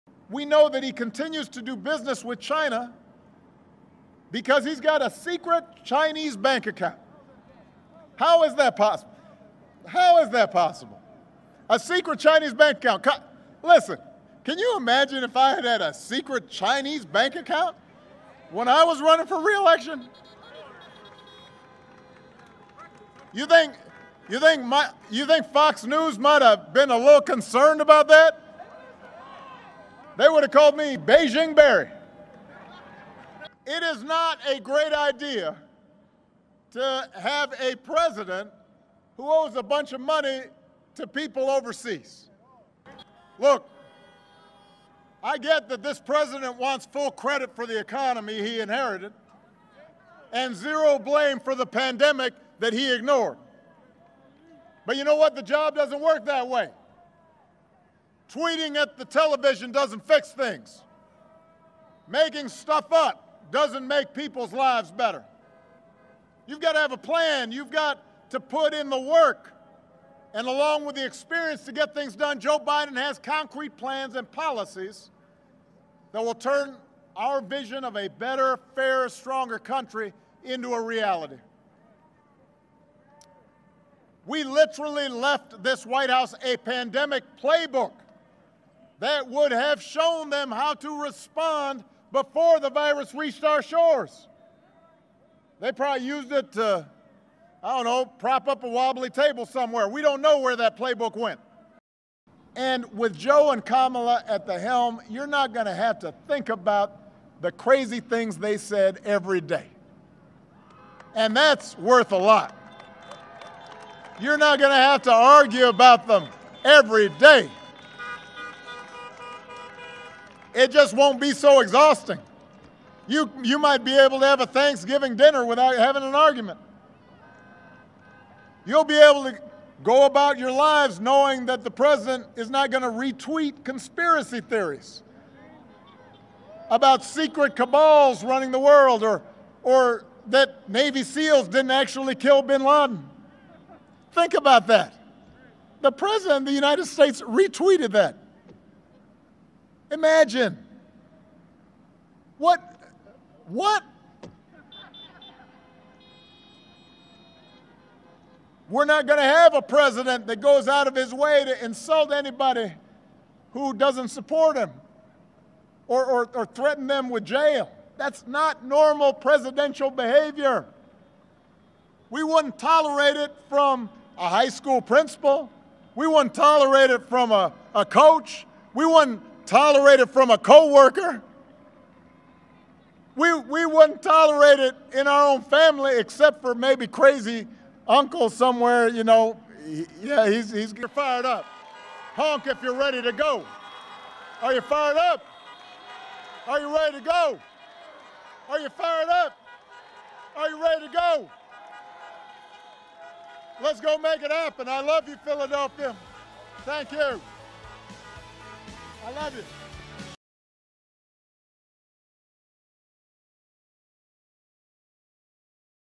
Barack Obama likens Donald Trump to a 'crazy uncle'
Barack Obama has delivered a stinging speech about President Donald Trump in a speech delivered in Philadelphia while campaigning for Joe Biden.

“Can you imagine if I had a secret Chinese bank account?” the former president asked, referring to a recent Times report, as he returned to the campaign trail in Philadelphia to stump for Joe Biden. ‘They would’ve called me Beijing Barry’

The former president criticised Trump's handling of the coronavirus crisis as well as divisive behaviour including retweeting conspiracy theories that you wouldn't tolerate from anyone “except a crazy uncle".